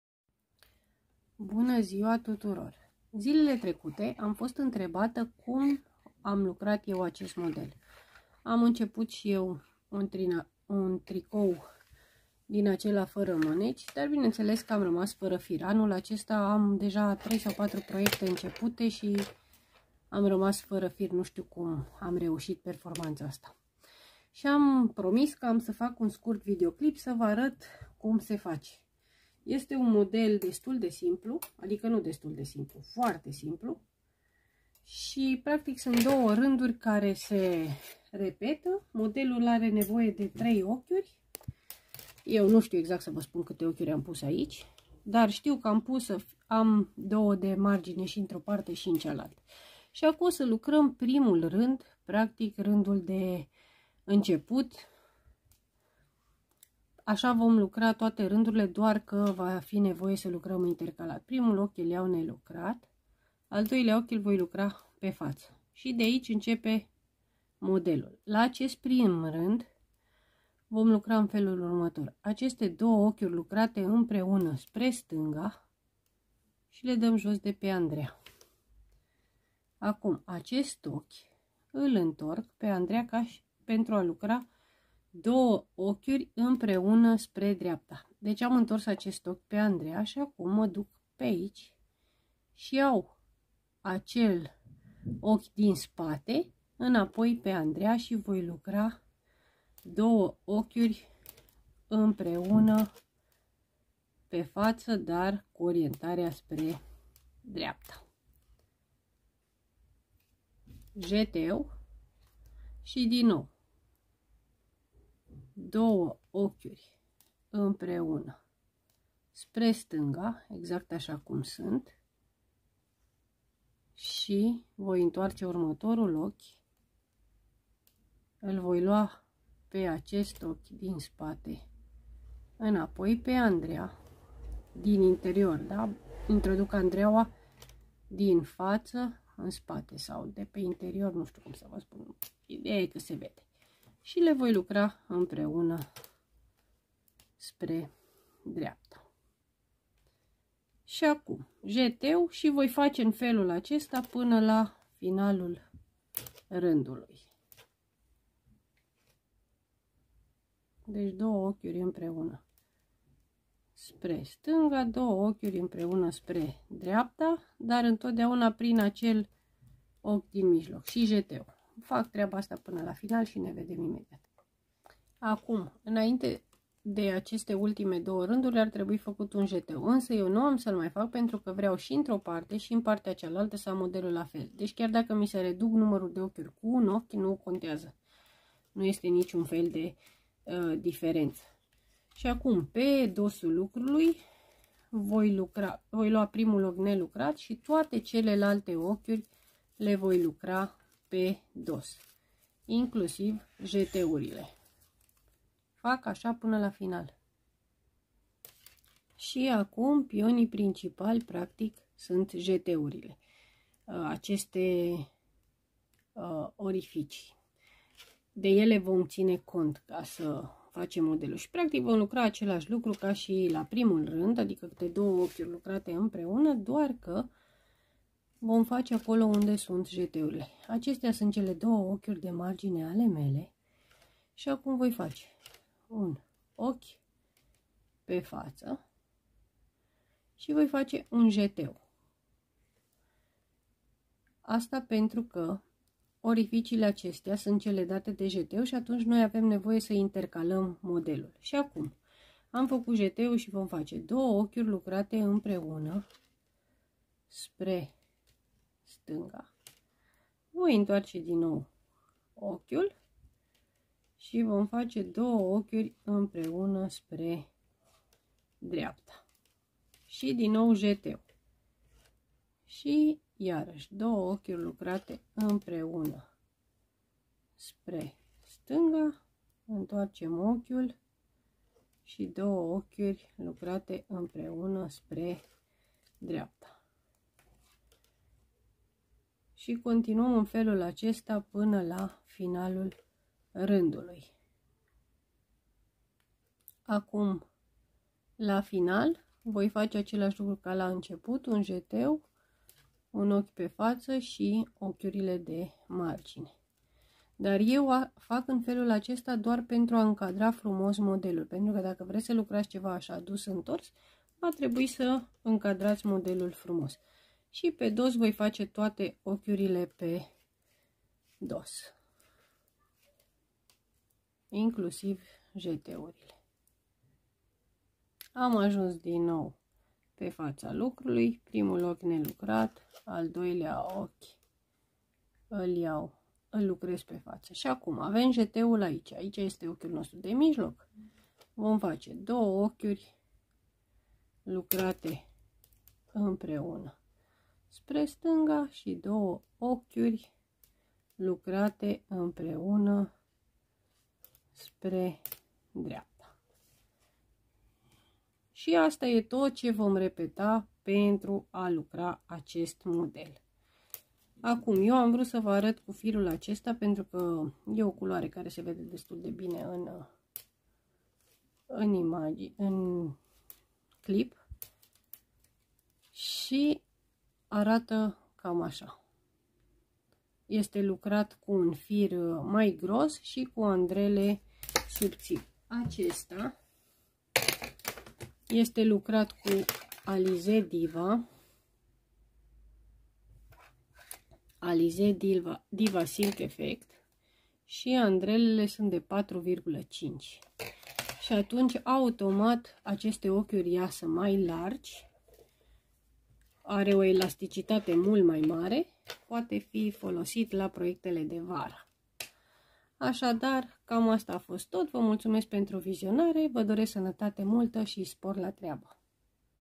Model tricotat -tutorial (un model potrivit pentru proiectele de vară,dar nu numai )
Bine ați (re)venit pe canalul meu !
Acesta este un model foarte drăguț, potrivit pentru proiecte de vară (dar nu numai)